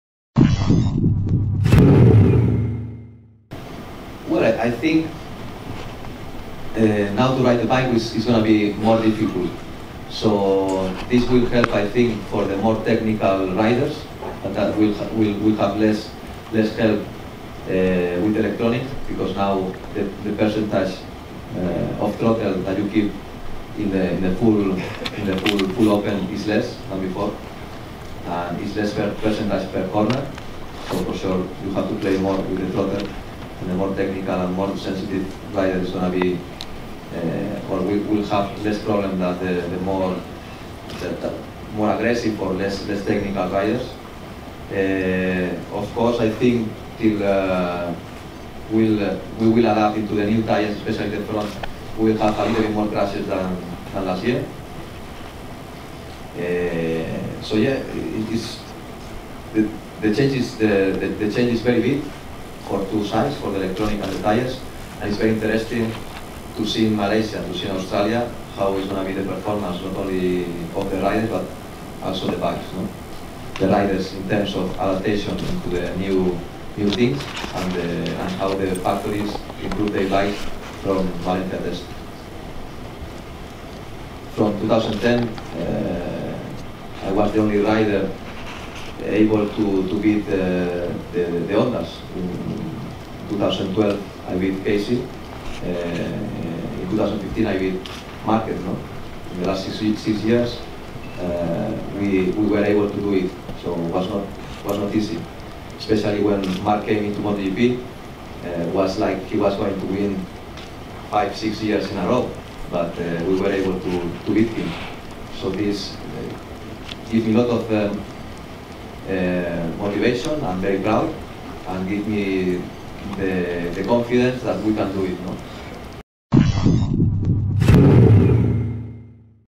Lorenzo: «Vincerò anche quest'anno»
Con le nuove gomme Michelin e l'elettronica unica la MotoGP 2016 promette di essere più impegnativa. Ma il campione del mondo in carica non ha paura: «Ho già battuto tutti e posso rifarlo»